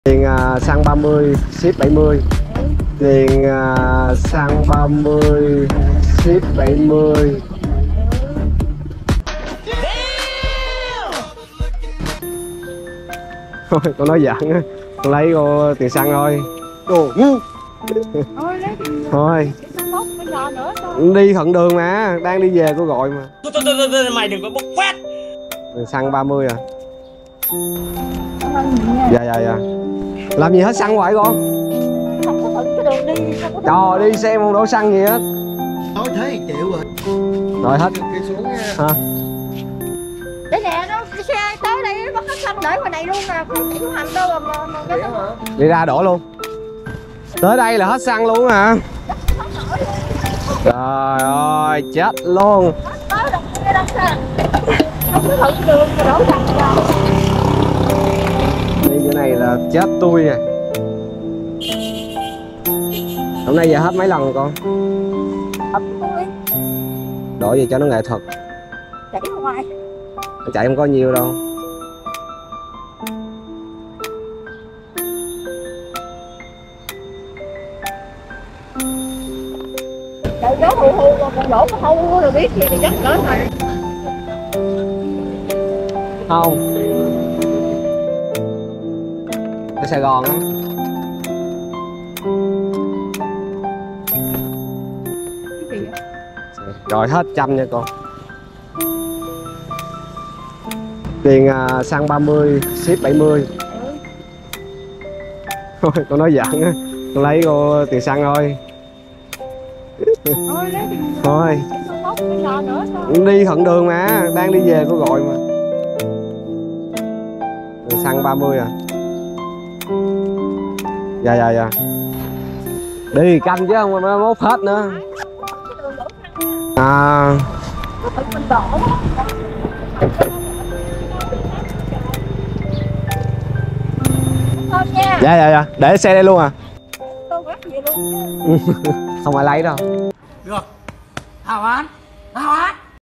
Anh grab mua xăng lấy phí ship khách cao ngất ngưỡng và cái kết!
Anh grab lấy phí ship cao nhất ngưỡng